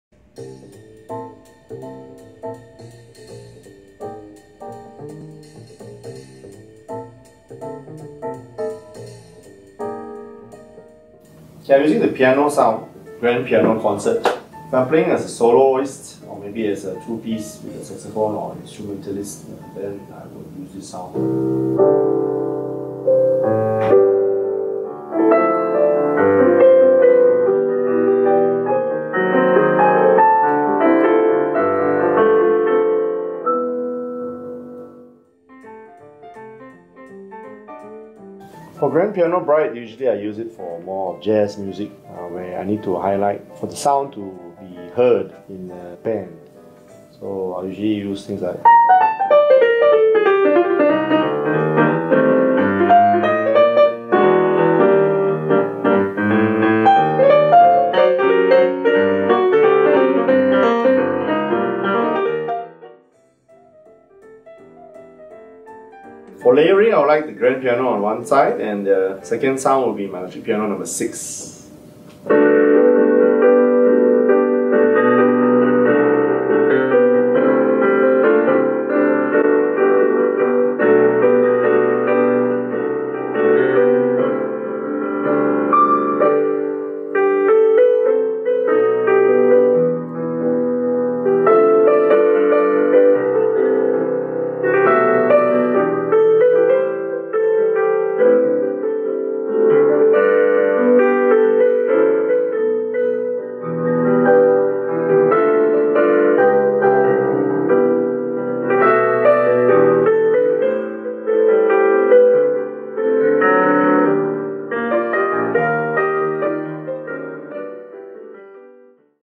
Casio PX-S3000 Review: Tone Demonstration for Grand Piano and Layering with Electric Piano
Marcus will demonstrates on different tone selection like Grand Piano Concert, Grand Piano Bright, and Layering with Electric Piano.